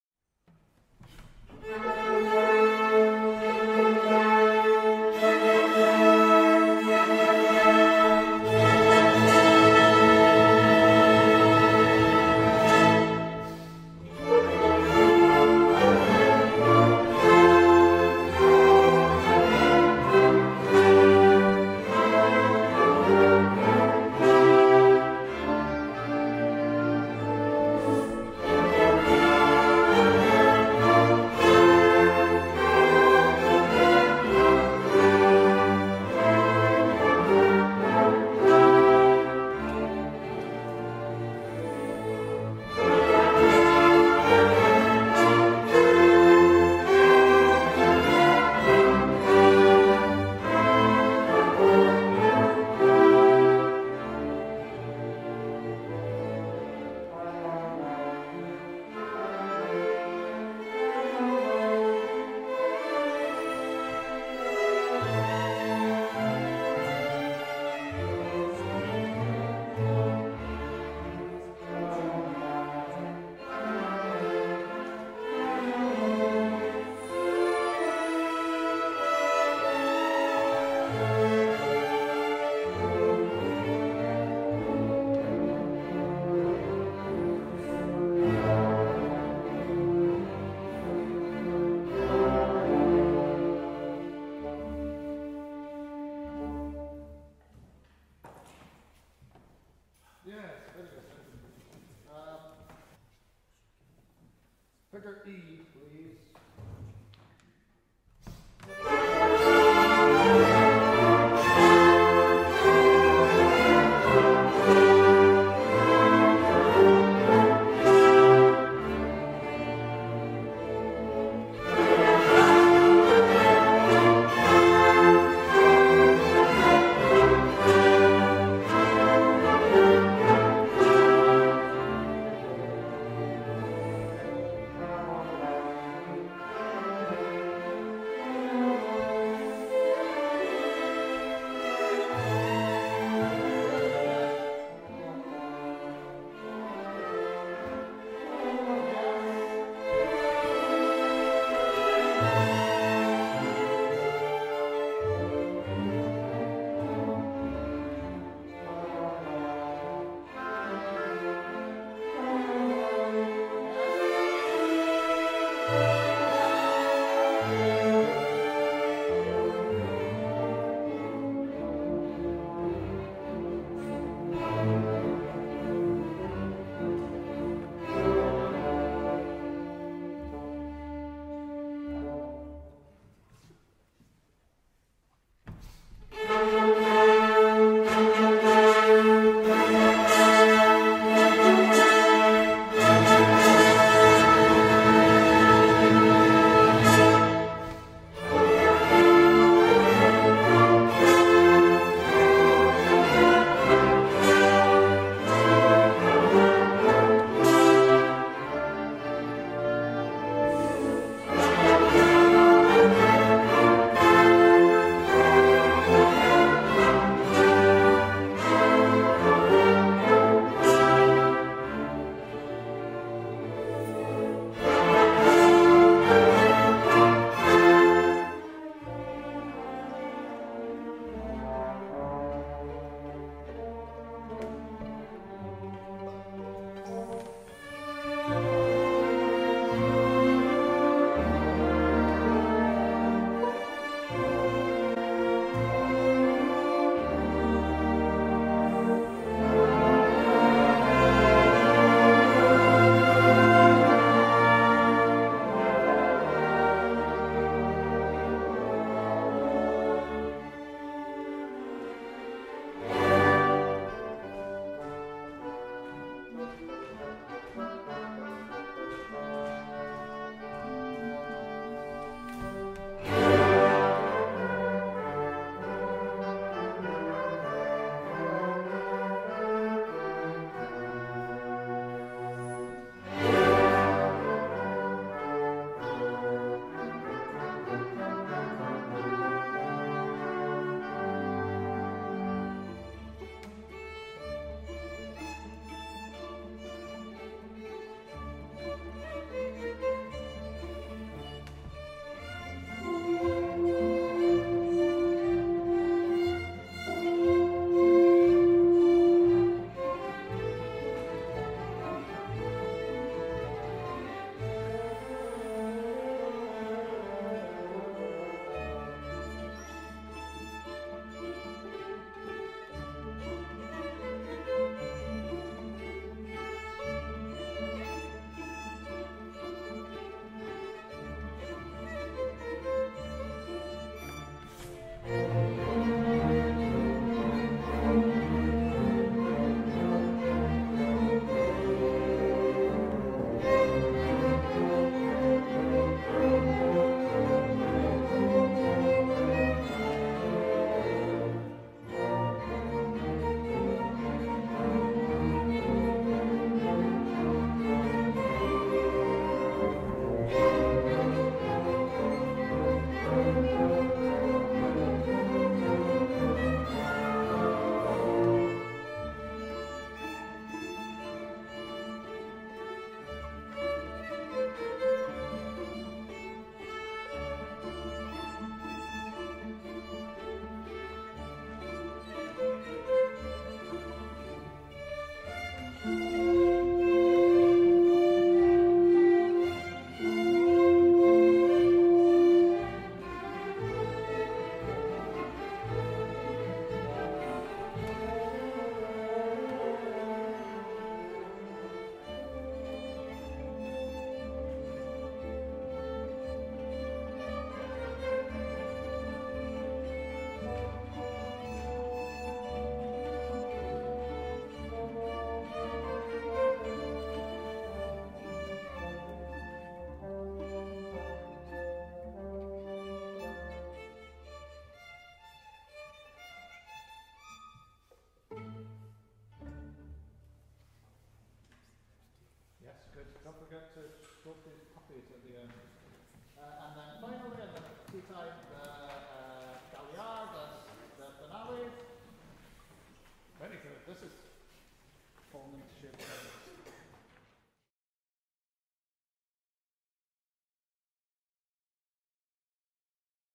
Learig Orchestra 2024
The Learig Orchestra of Aberdeen, Scotland in rehearsal in March 2024